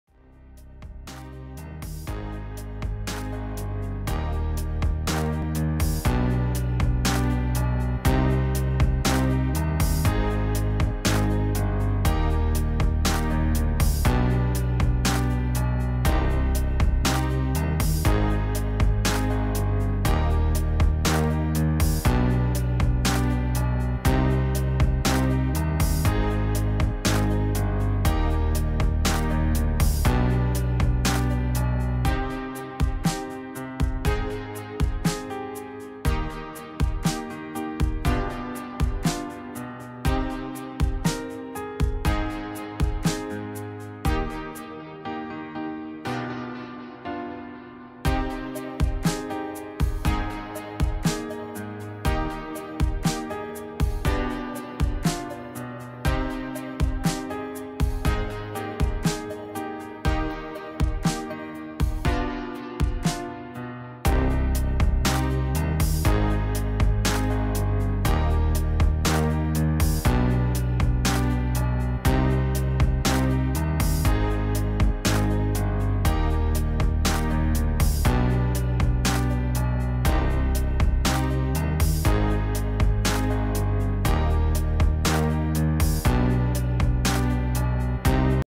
Vlog canada: Quebec, Montréal vue du ciel.
Montréal est la deuxième ville du Canada et la plus grande ville de la province du Québec,  une ville à visiter dans sa vie